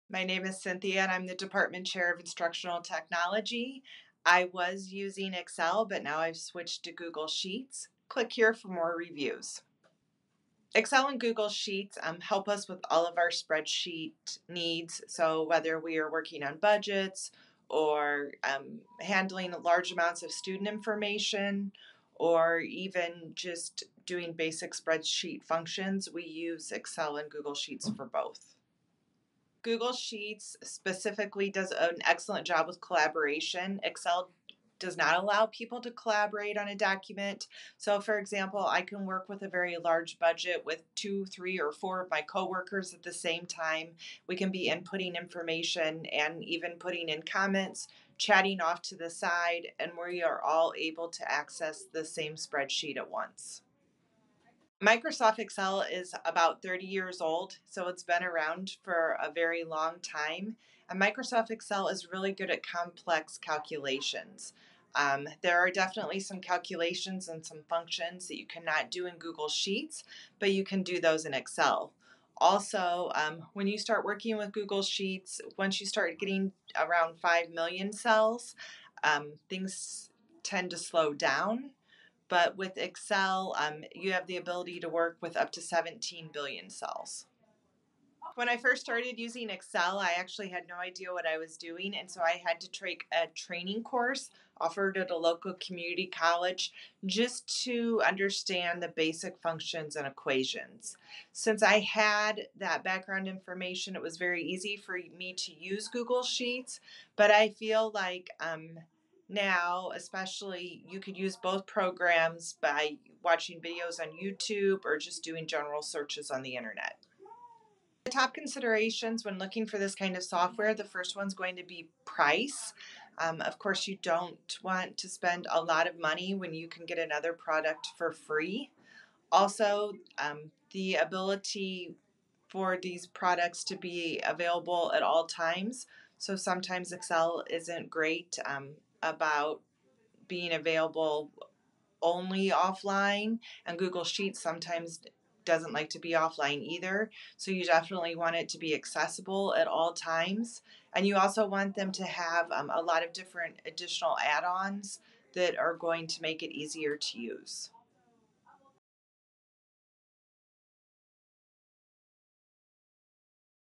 Google Sheets vs Microsoft Excel: Why they switched from Microsoft Excel to Google Sheets
🥊  Considering Google Sheets vs Microsoft Excel? Google Sheets vs Microsoft Excel Comparison: Features, Pricing and Integrations comparison.

💡📘 Interested in Google Sheets? Browse Google Sheets reviews, pricing and compare with popular alternatives

💡📘 Interested in Microsoft Excel? Browse Microsoft Excel reviews, pricing and compare with popular alternatives

Google Sheets Review 2021.

Cloud-based spreadsheet and collaboration platform.

Online spreadsheet making tool that helps in creating, editing & sharing spreadsheets in real-time through any PC, mobile or tablet.

Microsoft Excel Review 2021.

Spreadsheet software.

Leading spreadsheet and data analysis software. Typically bundled with Microsoft Office.

0:00 Intro
0:10 What business problems are you solving with Google Sheets and Microsoft Excel?
0:29 What is Google Sheets best for?
0:54 What is Microsoft Excel best for?
1:28 How easy is it to get started with Google Sheets and Microsoft Excel?
1:57 What recommendations do you have for those considering Google Sheets and Microsoft Excel?"